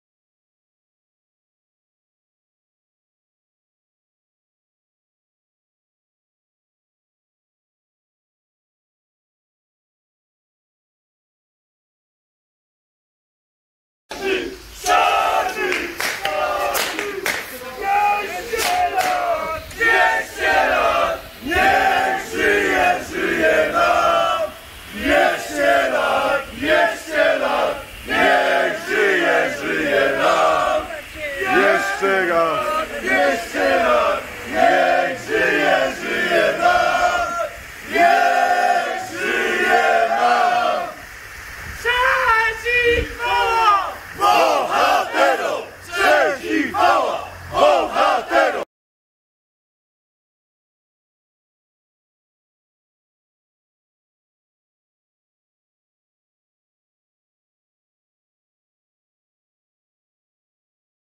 Narodowa Cracovia pod domem Pana Wacława Szaconia PS. ‘Czarny' | 05.02.2021
"W dniu wczorajszym udaliśmy się w kilkadziesiąt osób pod dom Pana Wacława Szaconia PS. ‘Czarny’, który świętował 95. urodziny. W asyście rac zaprezentowaliśmy okolicznościowy transparent i wręczyliśmy symboliczny prezent dla naszego bohatera. "